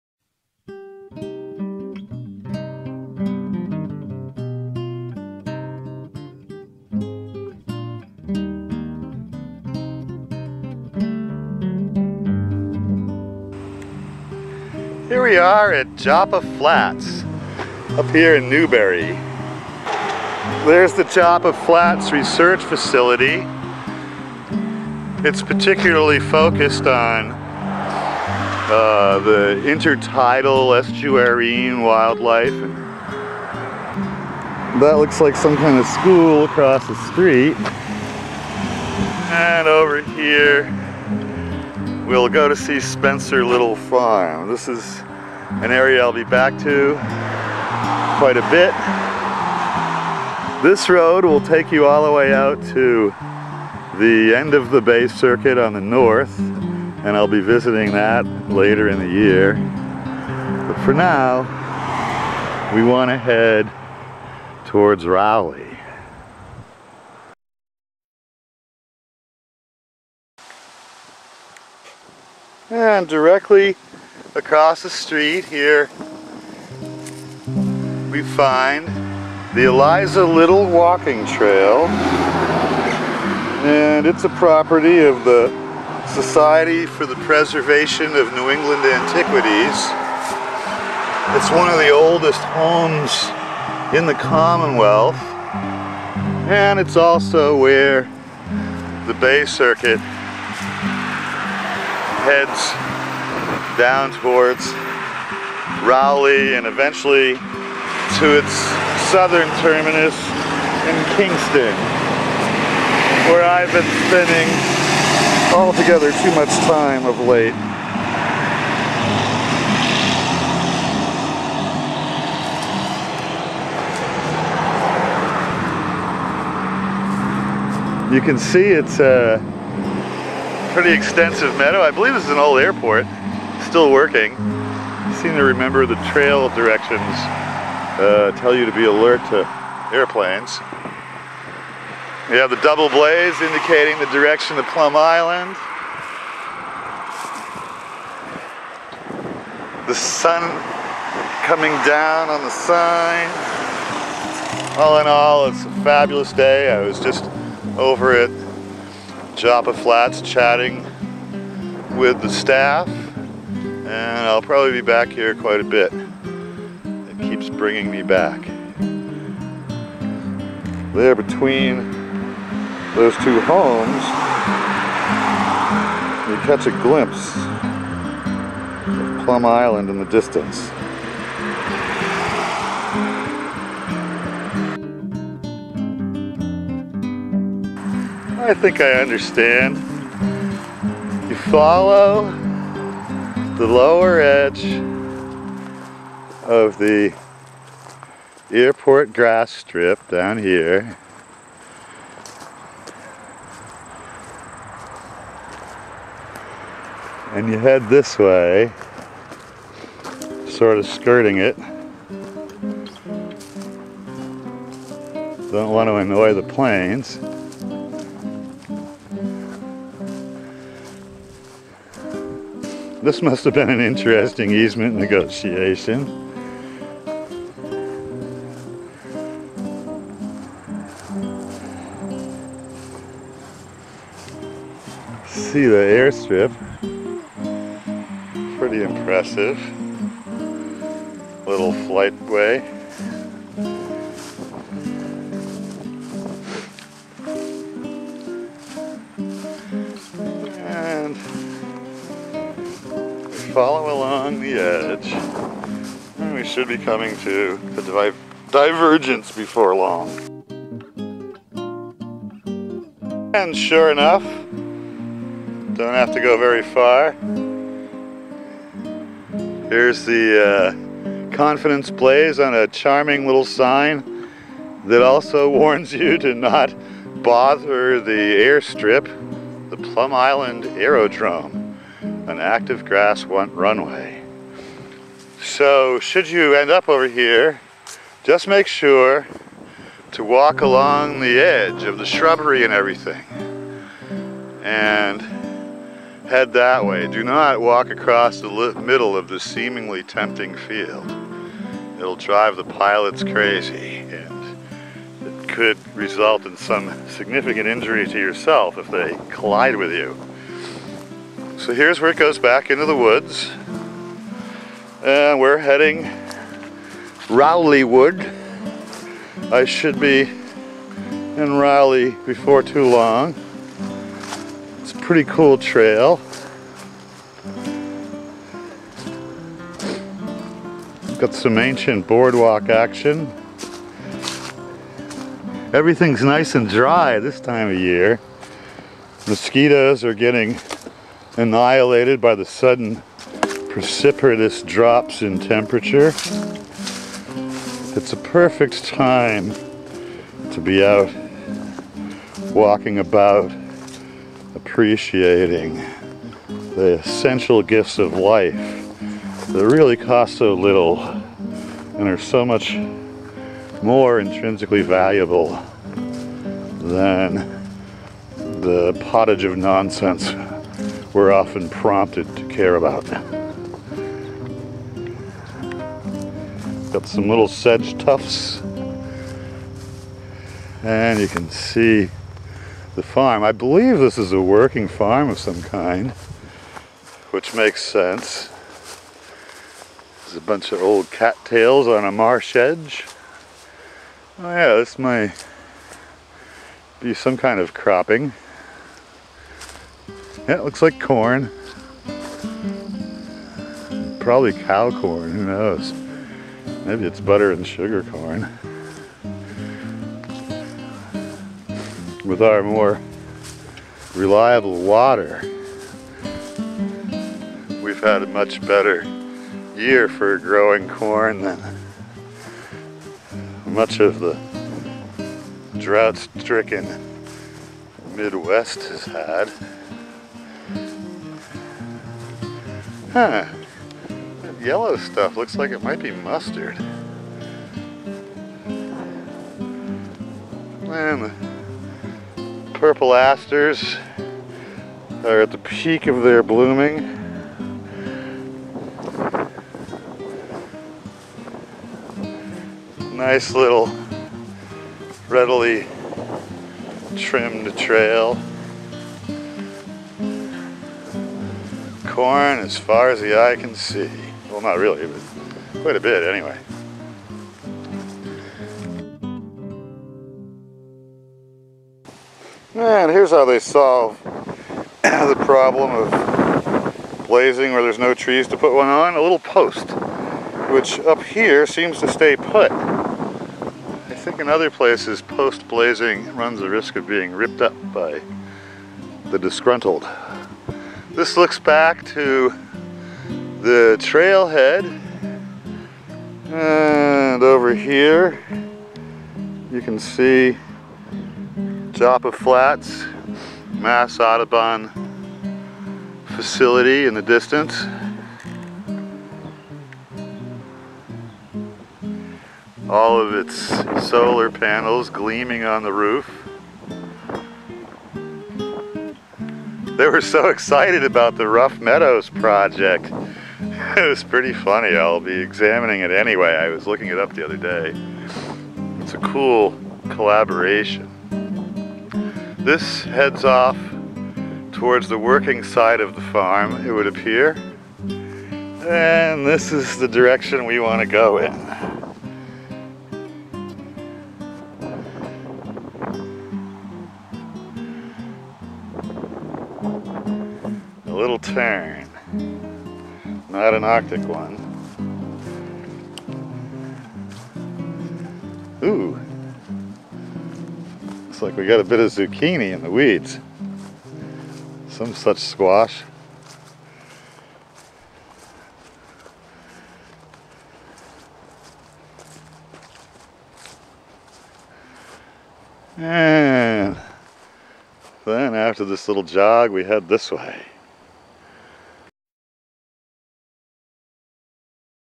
Bay Circuit Trail Newburyport MA: Joppa Flats Start Point.
This is the first of a set of clips from a hike from the Newburyport commuter rail stop to it's Rowley counterpart.

Joppa Flats is an education and research facility of the Massachusetts Audubon Society. The northern terminus of the Bay Circuit is just a ways down the road on Plum Island.